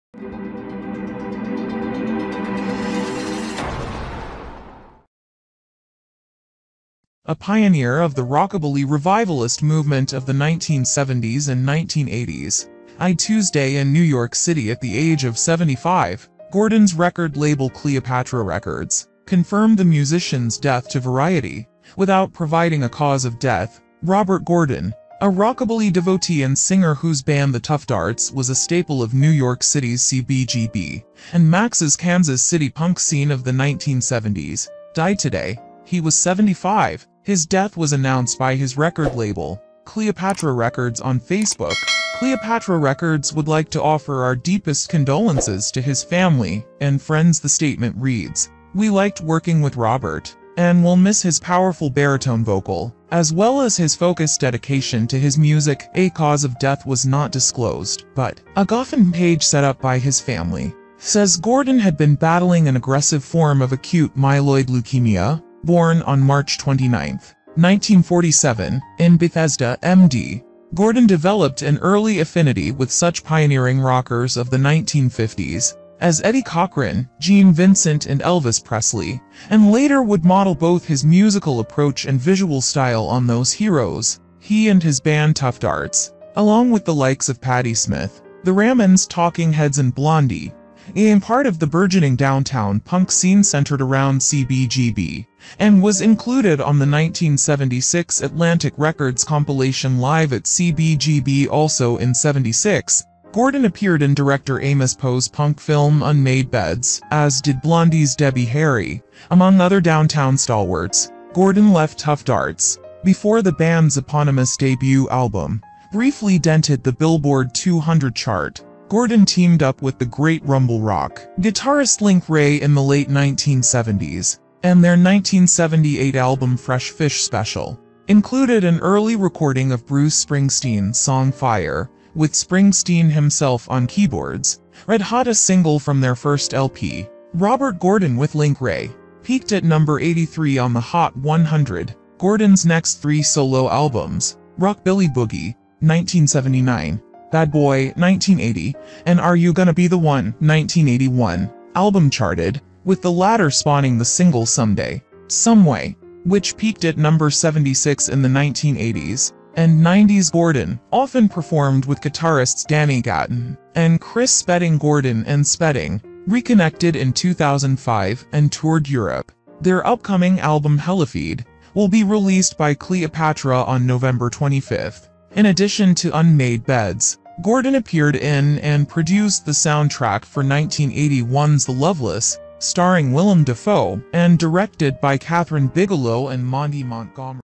American Singer Robert Gordon Passed Away After Hospitalization Dangerous Disease.
Robert Gordon was an American musician, best known as a neo-rockabilly singer.Robert Gordon, a rockabilly musician credited with helping to revitalize the genre in the 1970s, died on Tuesday following a battle with acute myeloid leukemia. He was 75. Cleopatra Records, the singer's label, confirmed the news in a statement released via Facebook, writing, "Rockabilly legend Robert Gordon passed away today. Cleopatra Records would like to offer our deepest condolences to his family and friends."